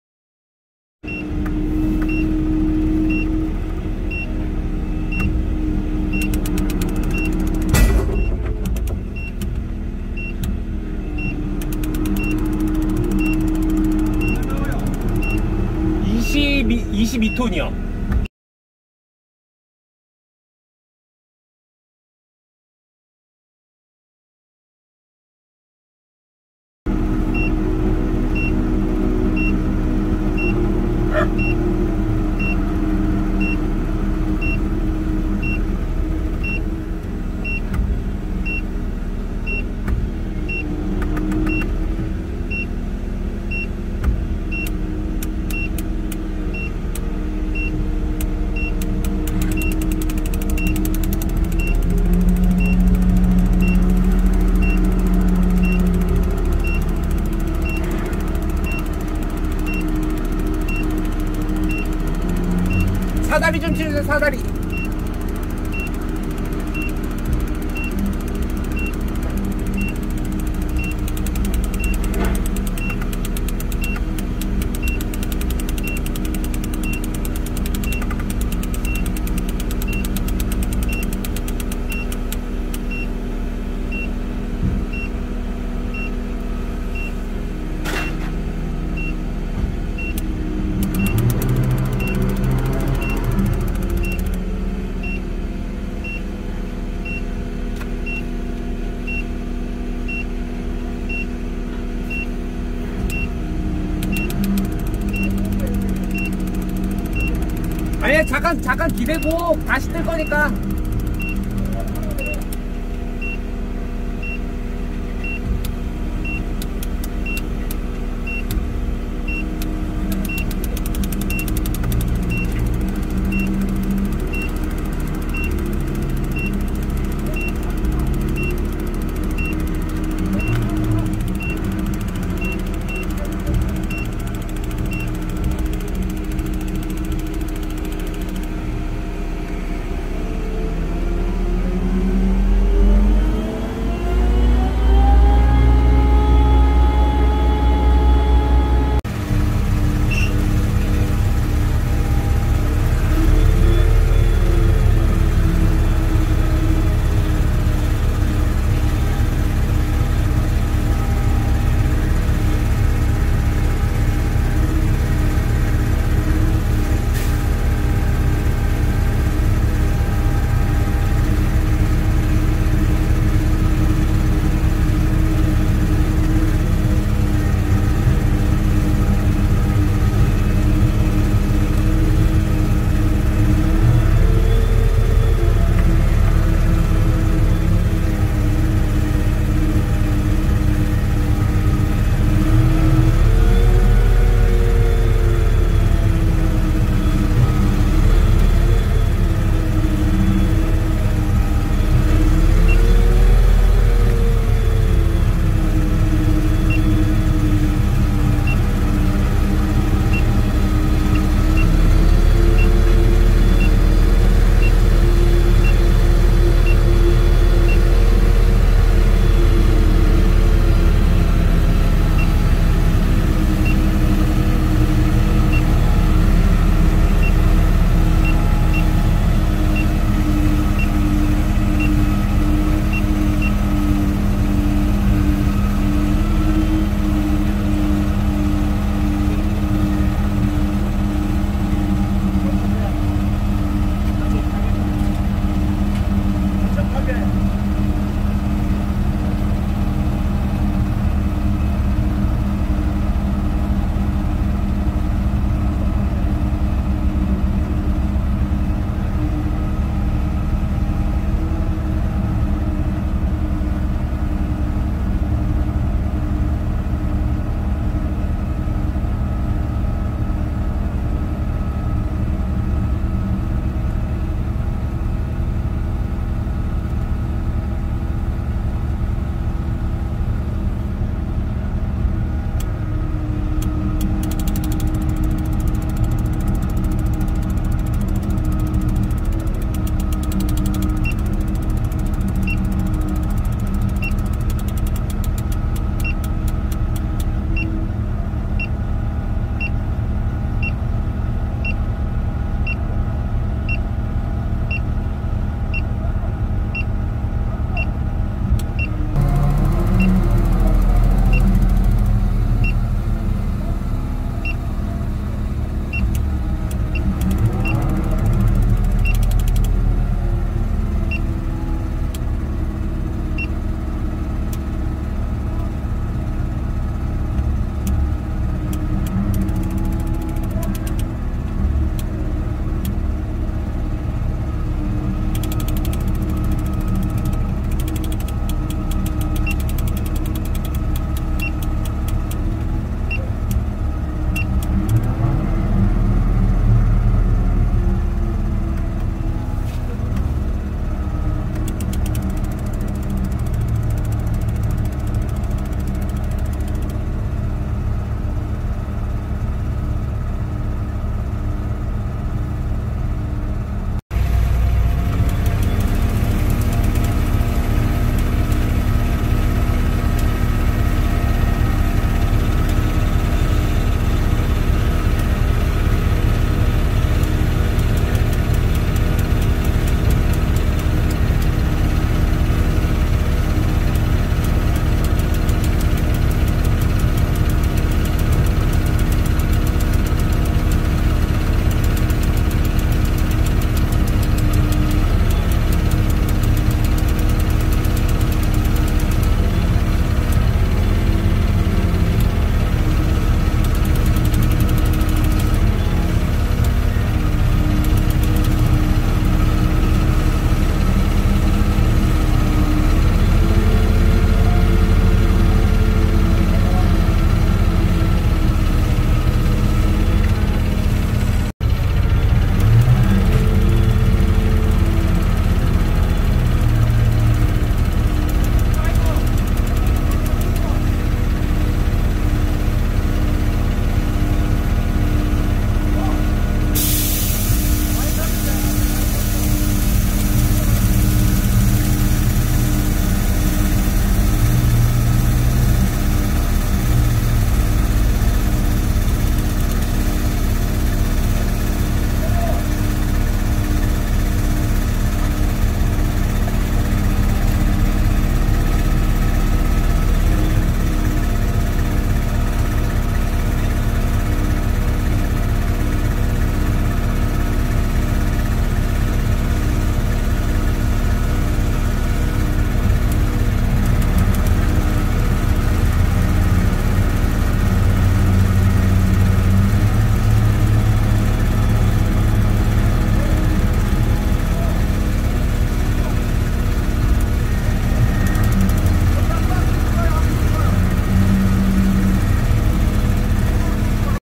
LTM1055 3.2 두번 다시 가기 싫은 그 현장,22톤 중량작업(해줄거 다해주고 단가로 딴지거는 작업자의 배려가 없는 그 현장)
야간작업을 하고 온 날 이어서 지역차 쓰시라 했는데 지역차 도망가고, 오후 02시부터 상차만 하기로 했는데 하루만에 작업 완료하려고 이동해서 하차까지 해줬는데 밥도 못 먹고, 사무실에서 가까워지는 이동거리도 아닌데 시간이 하루라고 하루치라고하네요.
어차피 상차하고 내일 하차하면 반나절 두번 사용하는건데.. 정정당당히 작업하고 정단가 고수하고 나왔습니다.